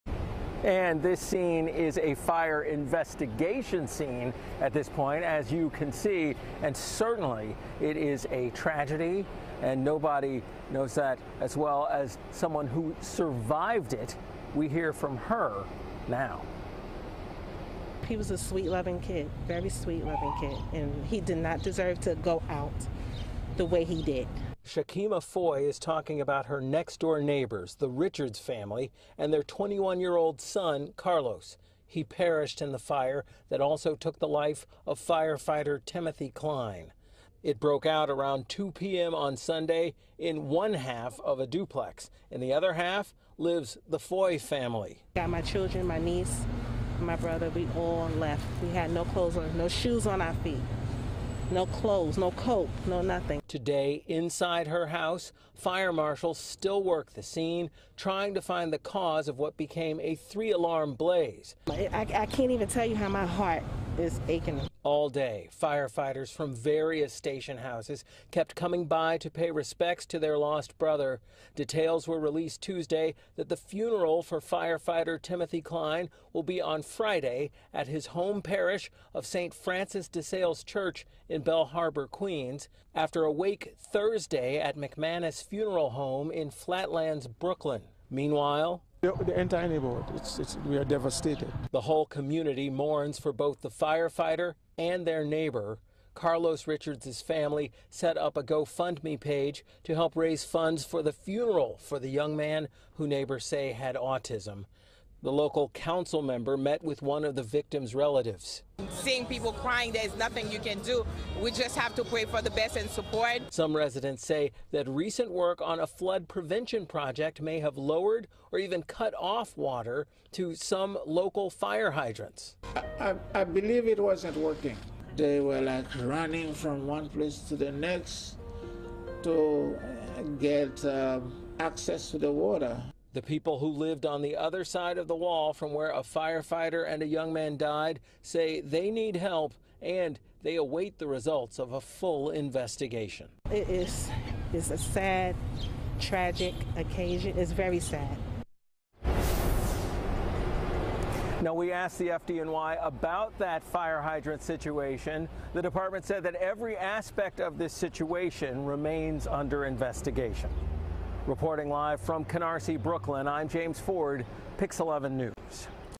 Survivors of fire that killed firefighter, resident give firsthand account
The fire scene in Canarsie, Brooklyn remains under investigation in the wake of the deaths of 31-year-old firefighter Timothy Klein and 21-year-old resident Carlos Richards. The intensity of the tragedy was recounted by people who lived it firsthand -- the family that lived in the other half of the duplex that went up in flames.